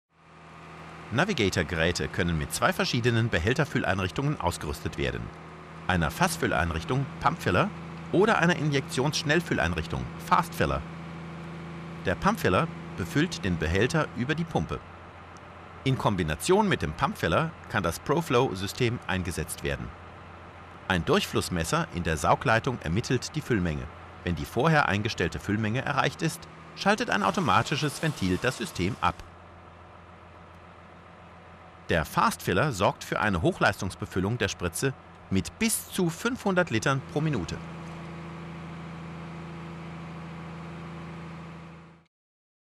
Filling / Befüllung - NAV (German)
HARDI NAVIGATOR - NAVIGATOR NAVIGATOR Geräte können mit 2 verschiedenen Behälterfülleinrichtungen ausgerüstet werden.
Als Anschluss wird eine robuste Aluminiumkupplung verwendet.